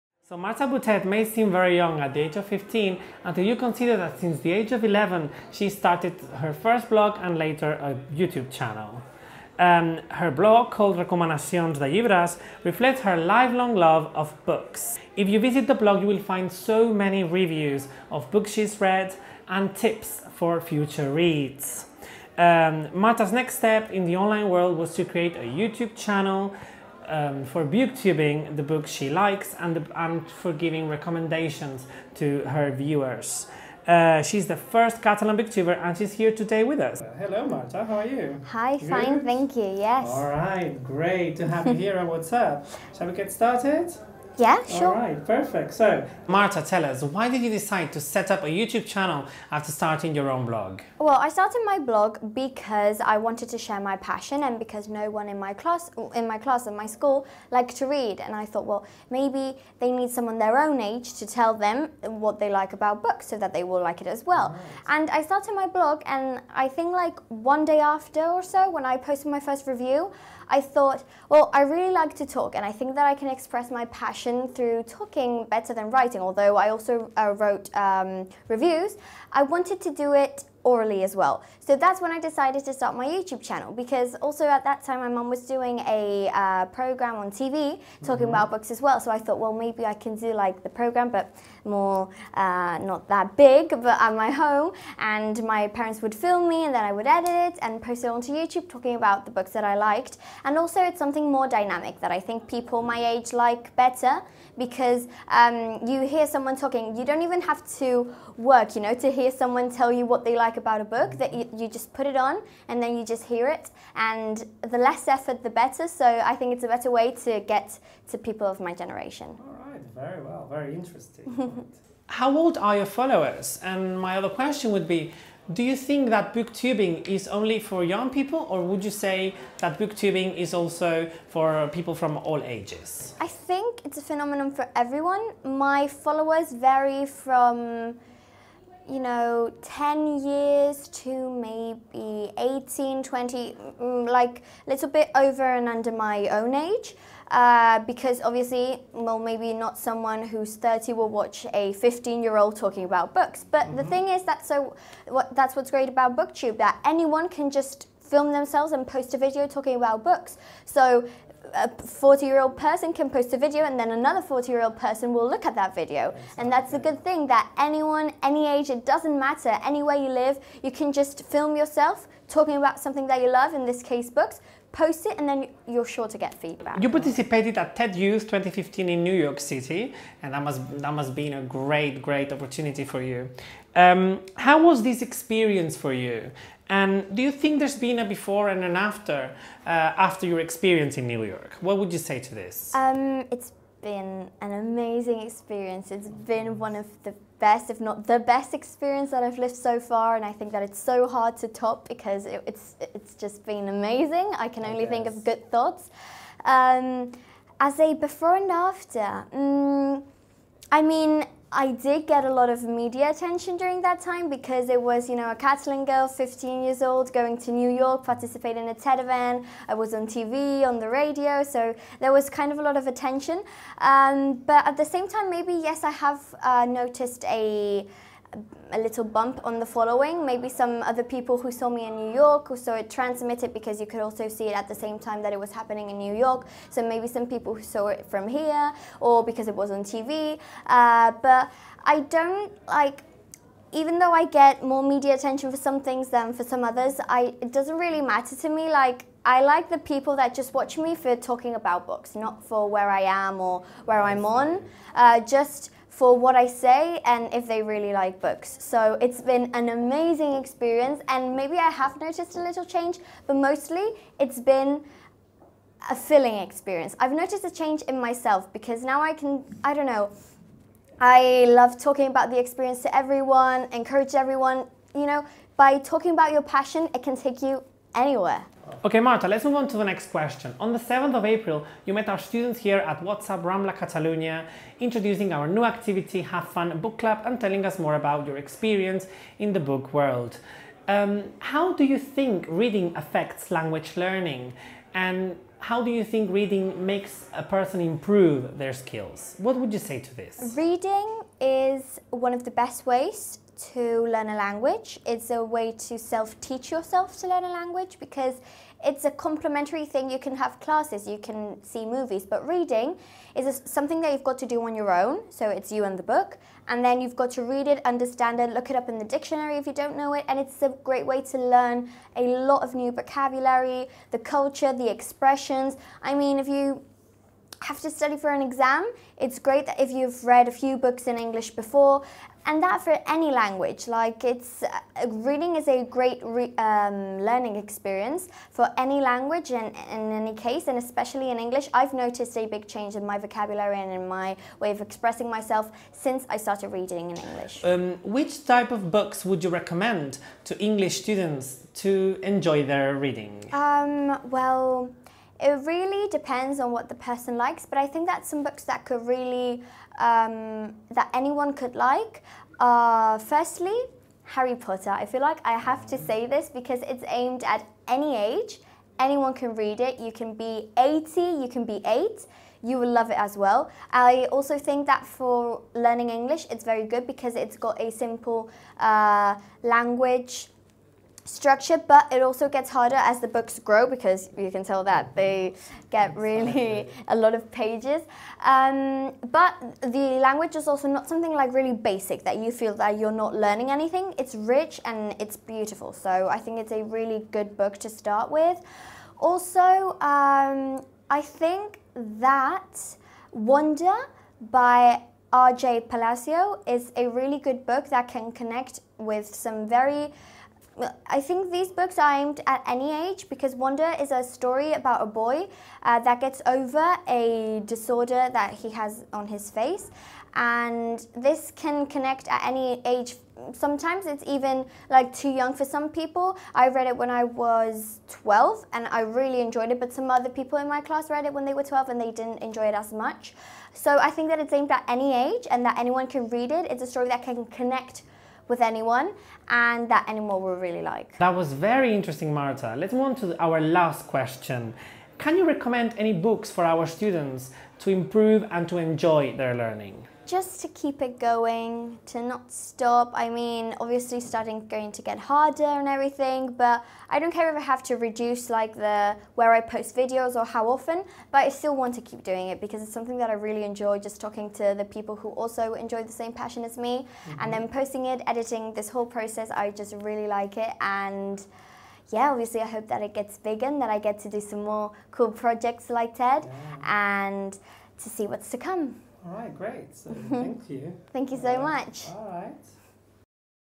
What's Up! Rambla Catalunya | Guest speaker | Interview with Marta Botet
La Booktuber Marta Botet vino a What's Up! Rambla Catalunya como invitada para hablarnos sobre su experiencia en TED Youth y su pasión por la lectura. Con ella iniciamos la nueva temporada de Book Club. Aquí puedes ver la entrevista que nos concedió antes de su charla.

Producción y Edición: Eric Dexeus